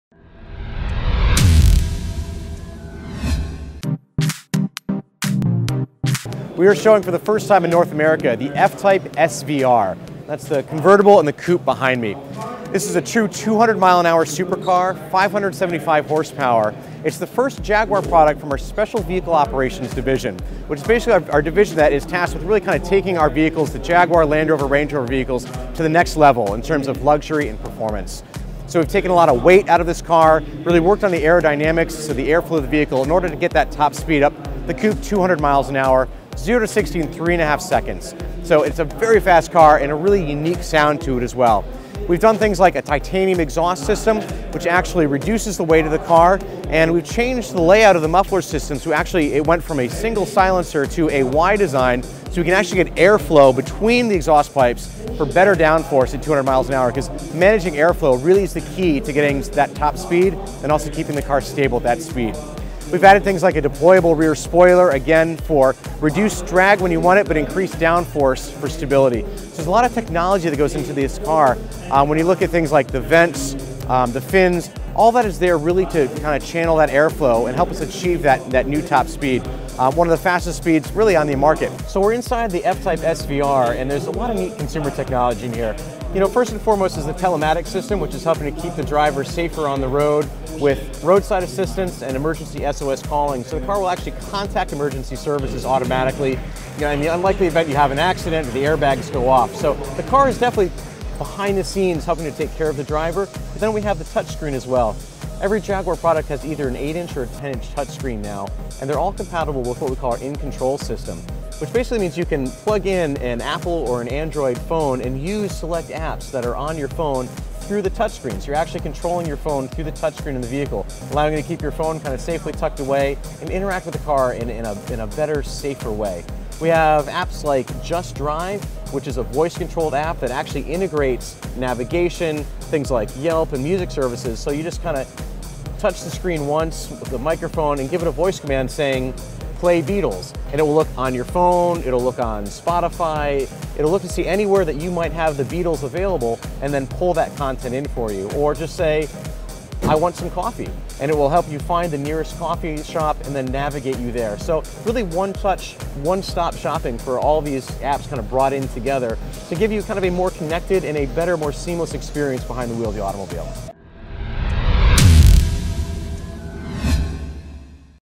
Zero To 200 In Jaguar's F-Type SVR
The F-Type SVR, Jaguar's most muscular car, is also the auto-maker's most connected car. Not only will the 575 hp SVR go 0-60 in 3.5 seconds and reach 200 mph -- it'll also contact emergency services automatically and deliver you to the nearest cup of coffee. Read the story in Tech Times